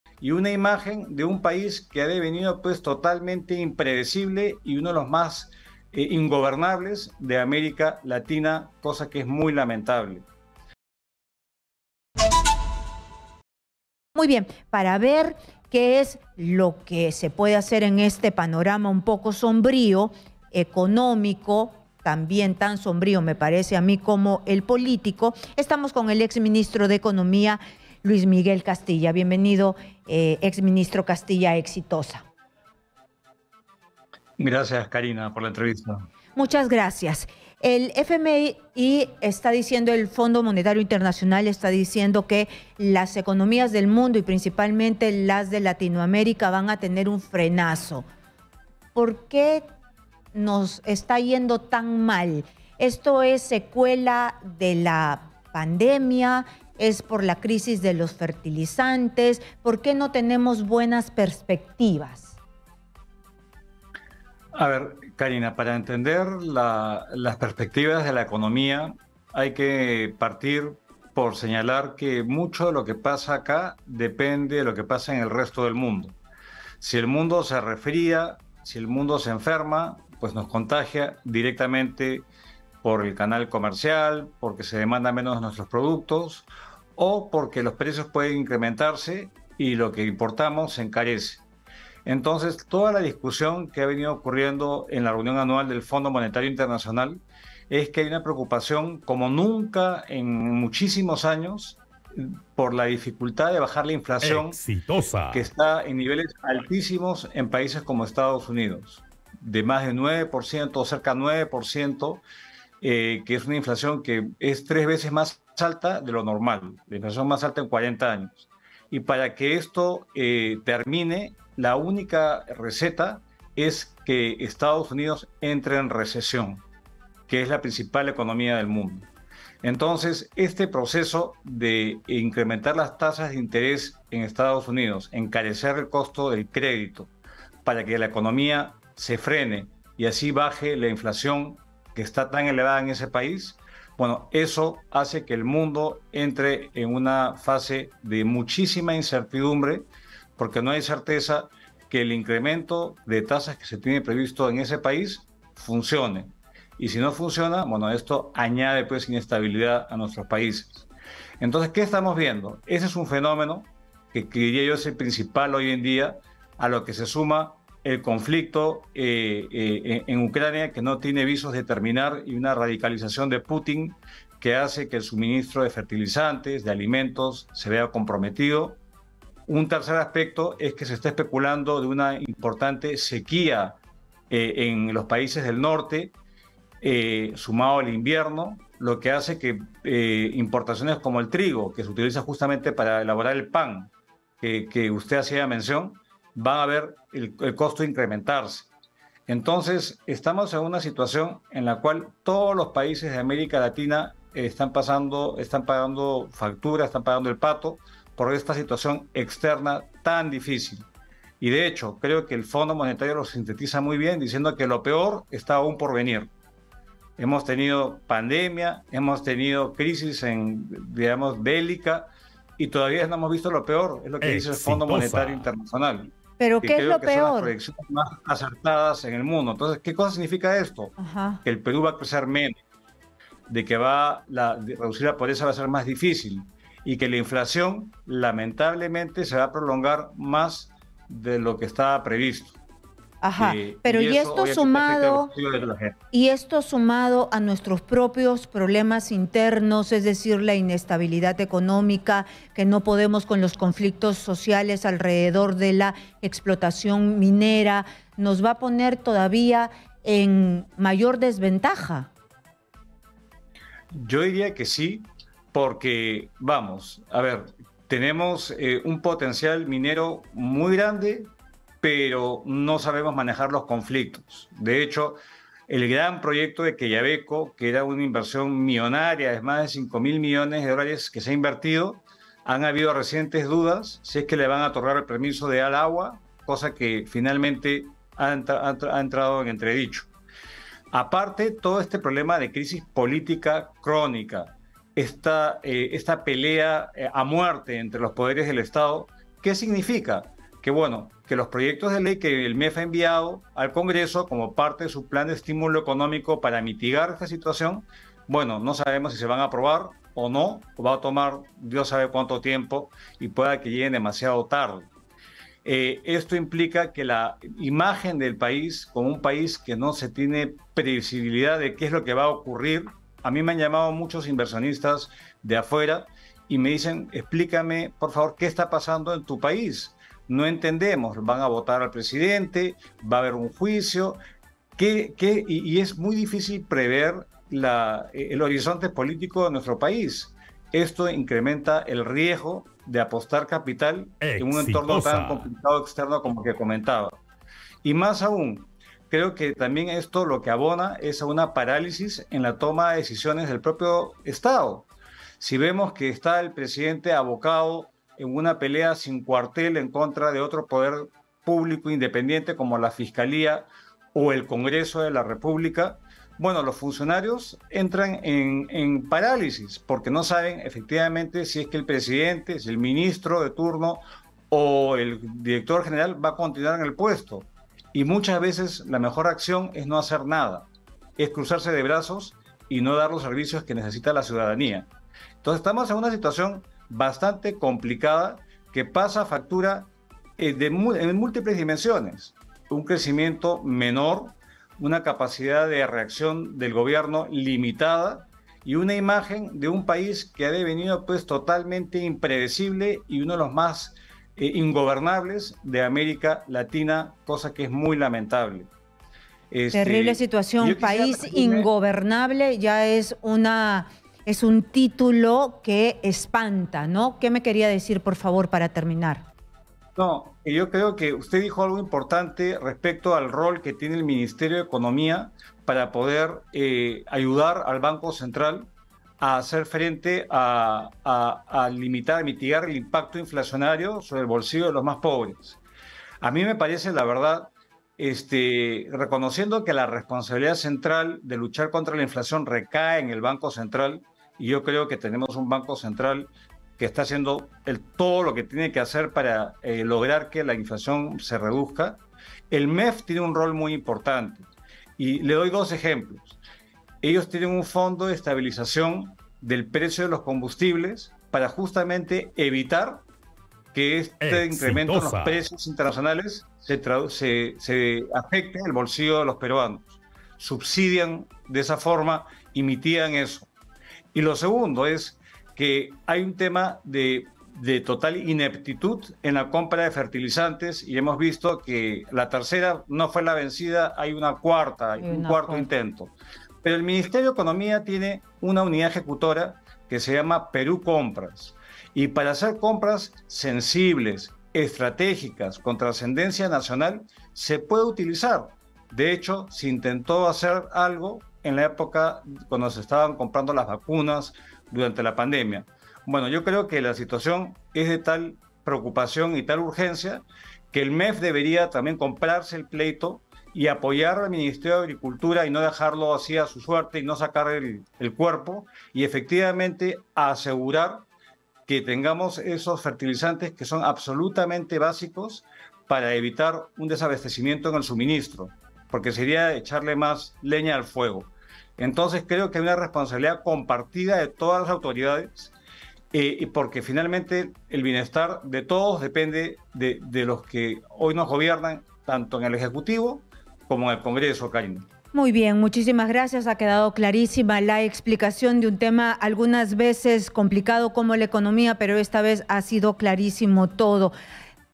🔴🔵Luis Castilla: "Perú es uno de los países más ingobernables de América Latina"
En entrevista con Exitosa, el exministro de Economía, Luis Miguel Castilla se pronunció sobre la economía y política en el Perú. "Perú es uno de los países más ingobernables de América Latina", indicó.

Exitosa Perú
"La voz de los que no tienen voz"

Corporación Universal